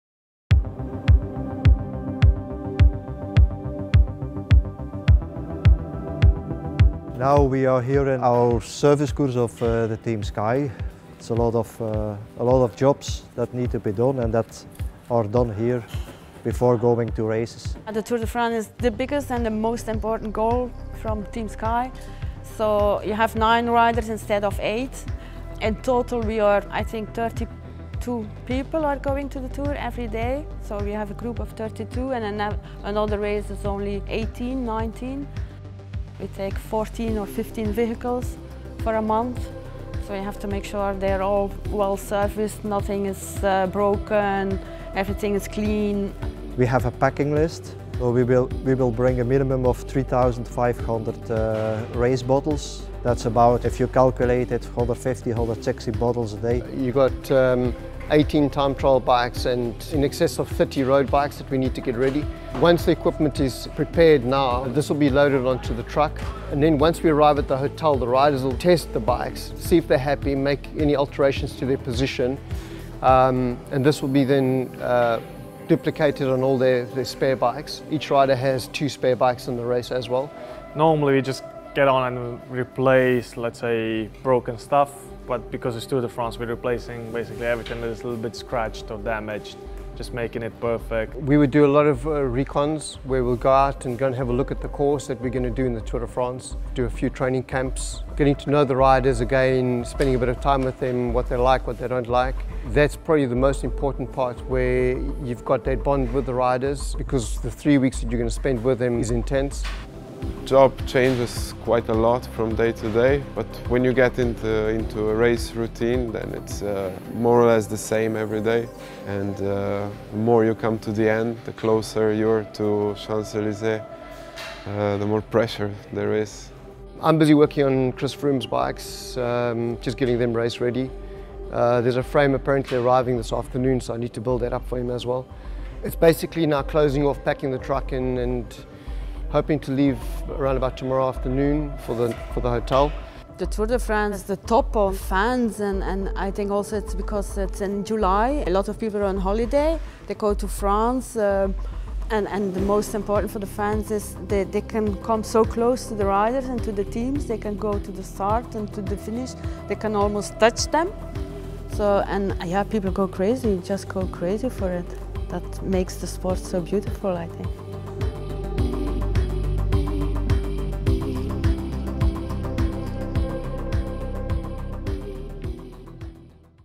Ford: How Team Sky preps for the Tour de France 2016
Ford headed to the Team Sky Service Course in Belgium to see all the work that goes into getting ready for the world's biggest bike race